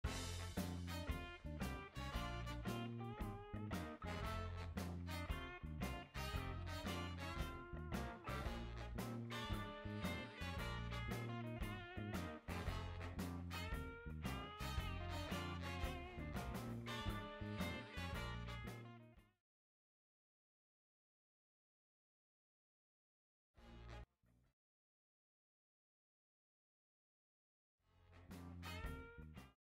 Speed PS 月上树梢头
魅力夜景制作，可爱猫咪观望